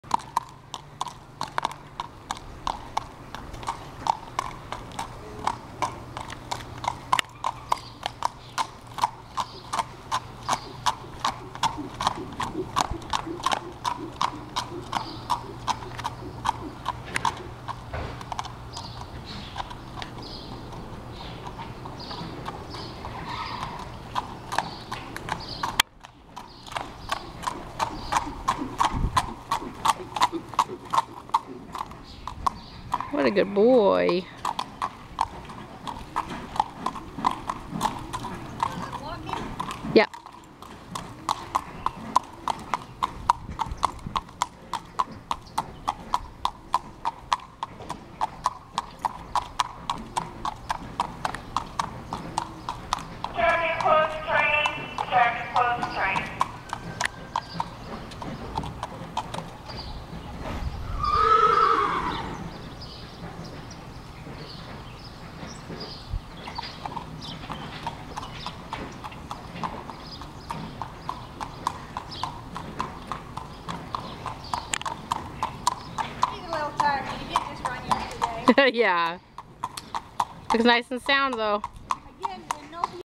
FOR SALE: Leroy- 5yo 16.3h gelding by Smart Chip. $2500
Please contact Katie at Hidden Fox Farm